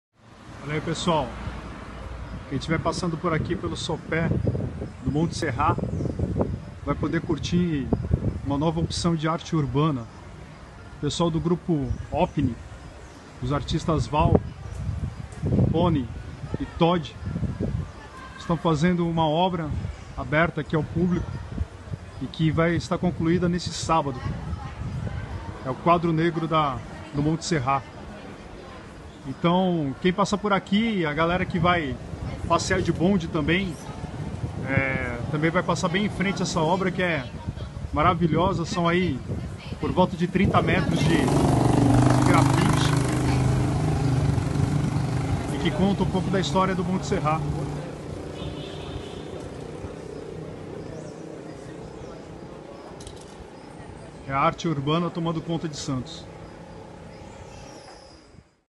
Grafite colore sopé do Monte Serrat
Um muro com cerca de 30 metros de comprimento, na Rua Bittencourt, no sopé do Monte Serrat, transformou-se em imenso painel que mostra detalhes de um dos principais cartões-postais de Santos. Saiba mais